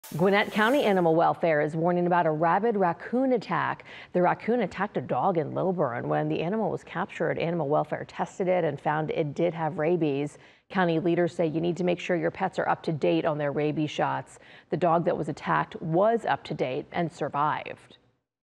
Rabid raccoon attacks dog in Gwinnett County, community services department says
Gwinnett County officials are urging the public to be cautious of oddly behaving animals after a confirmed case of rabies.